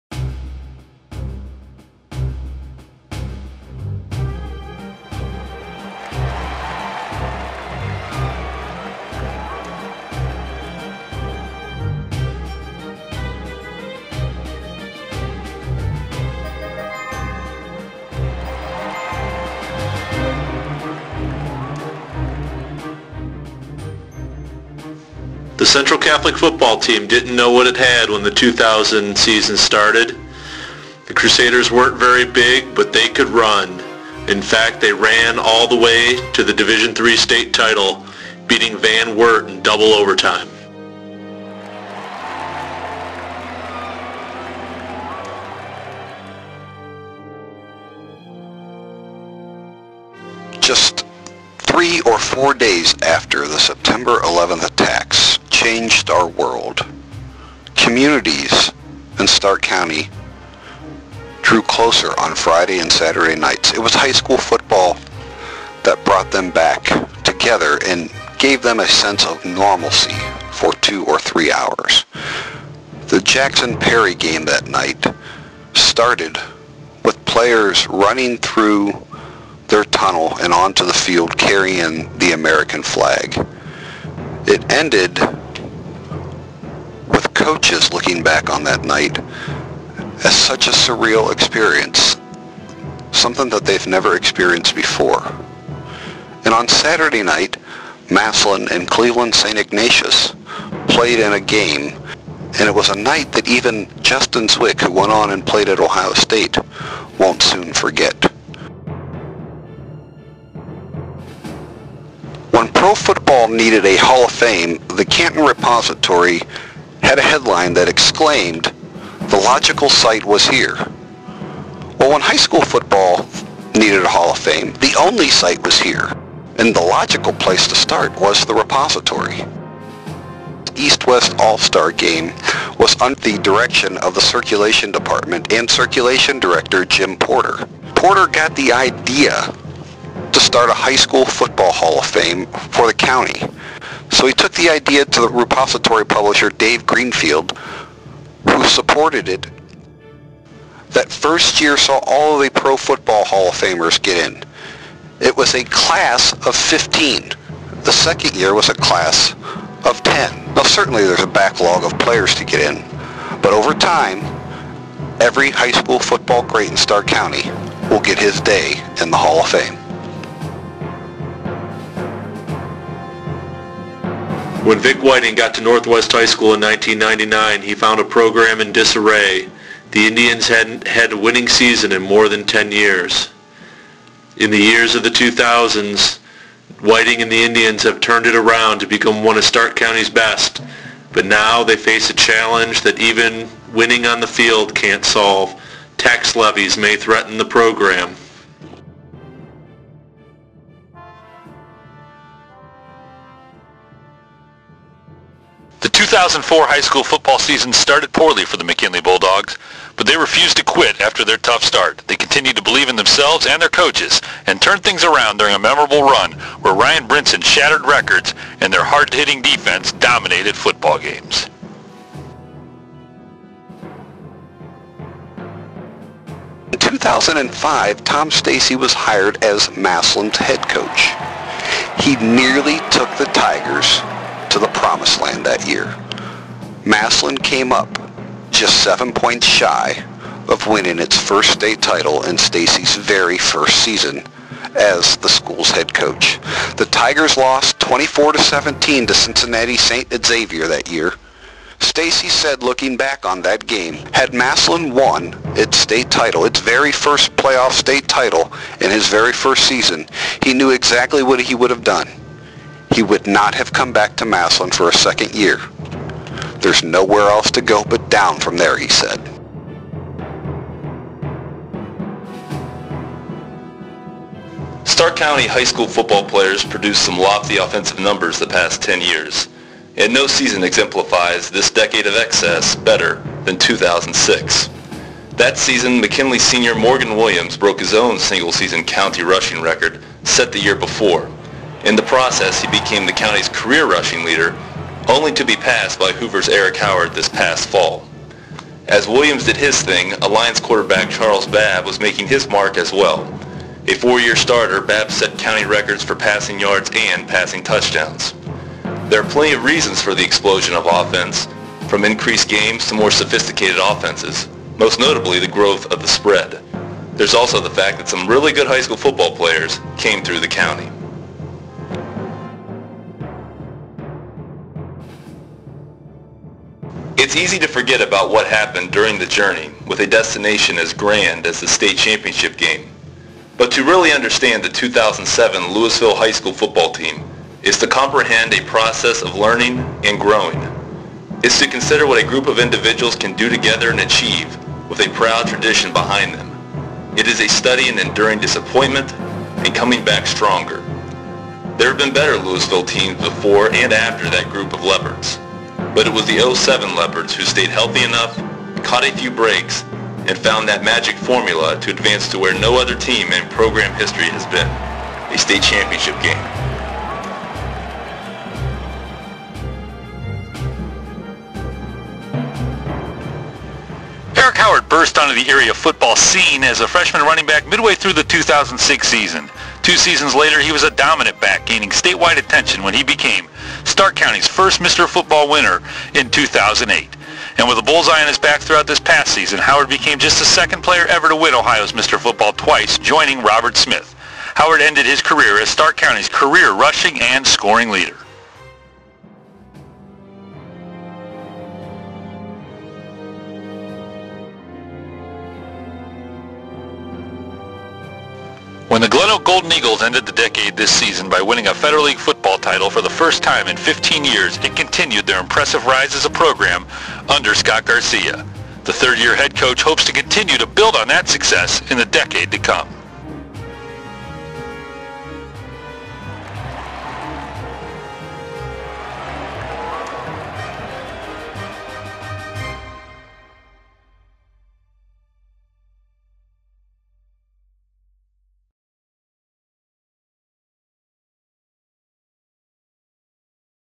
High School Football: The decade in review
Stark County high school football: The decade in review.
The Repository sports staff narrate the past ten years in Stark County football.